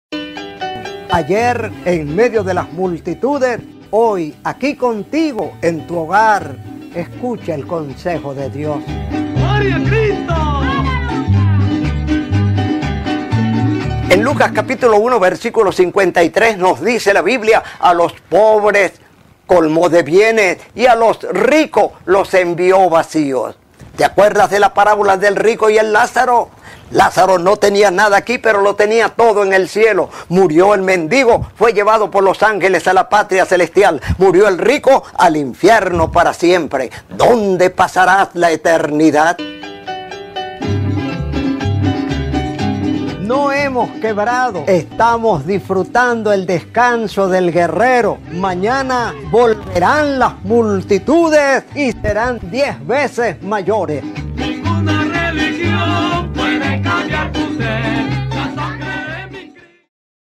HNO PUERTAS #141 | A LOS POBRES COLMO DE BIENES | PRÉDICA BÍBLICA | MICRO HNO PUERTAS
Dios tiene vienes preparada una cantidad enorme de bienes para ti, sin importar lo que hoy en día tengas

pastor Jaime Puertas:
"A los pobre colmó de bienes"